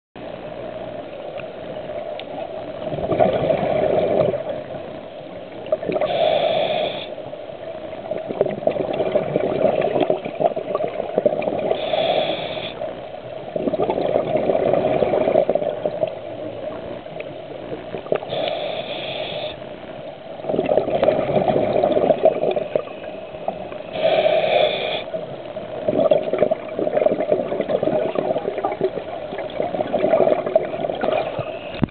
Barracuda
Un barracuda nel mare di Antigua